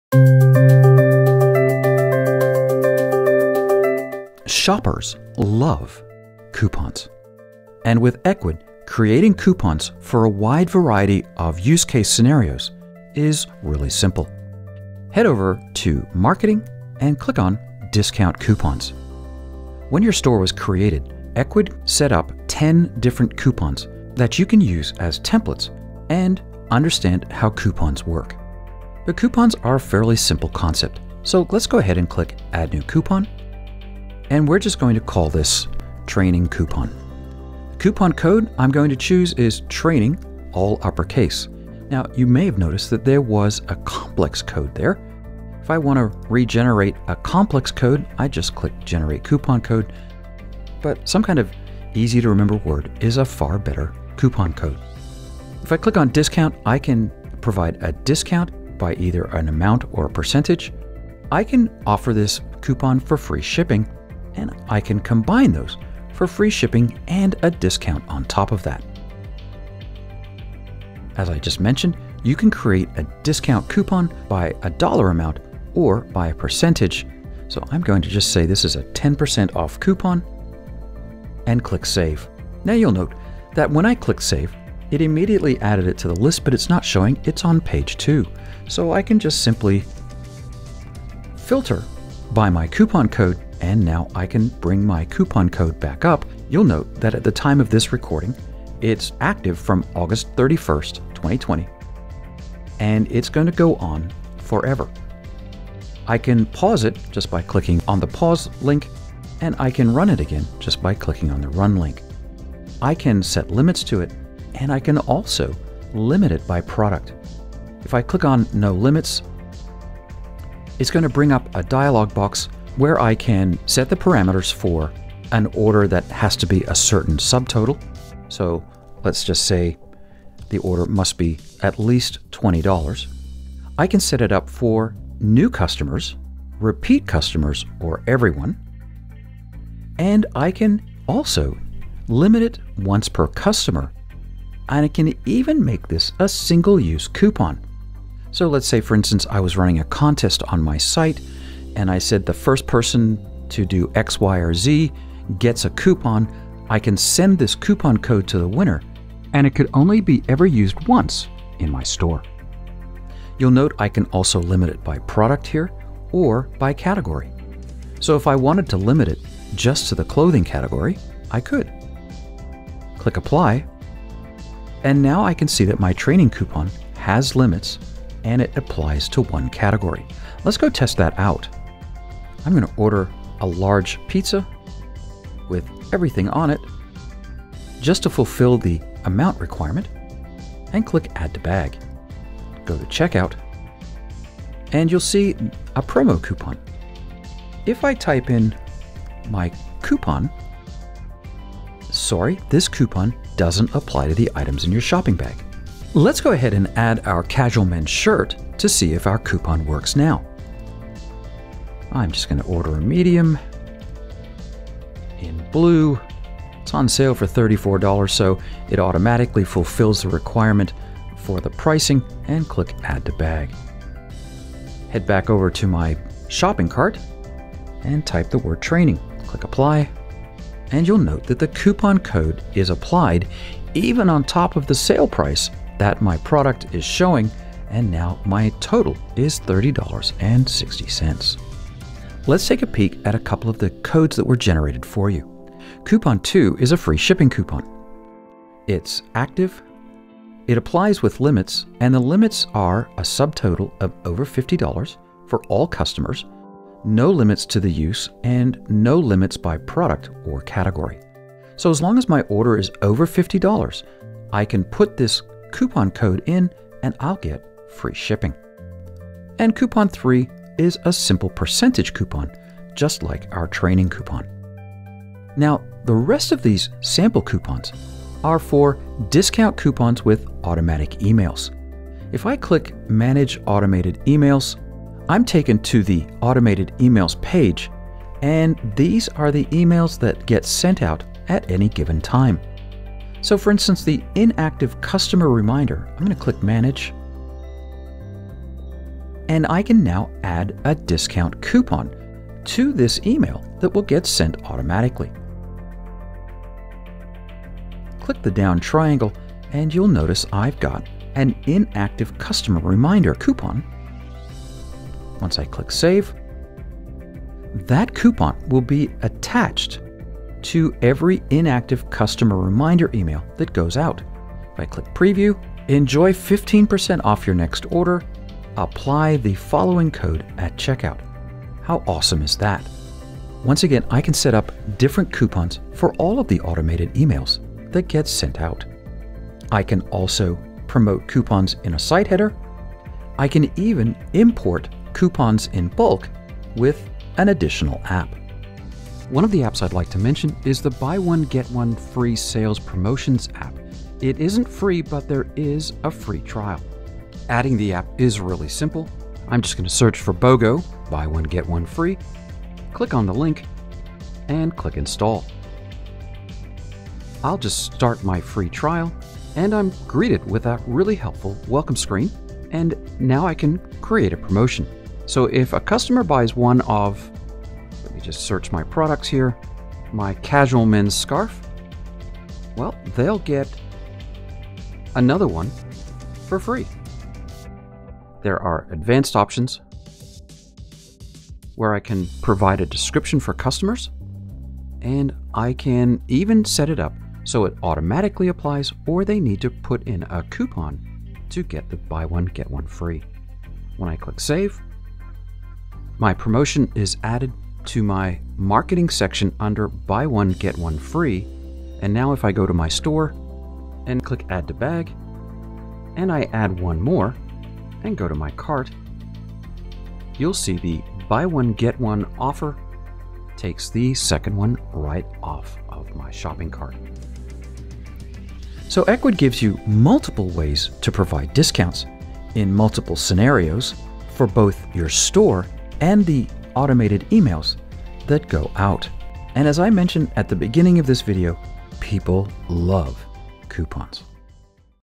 How to: Create discount coupons - Ecwid E-commerce Support
0:00 Intro
0:15 Add a new coupon
1:42 Setting limits on discount codes
3:02 Test a coupon code on checkout/example codes
4:35 Discount codes for marketing emails
5:48 Buy One, Get One Free app overview
7:08 Closing

📘Want to learn more information on this topic? Read this article in our Ecwid Help Center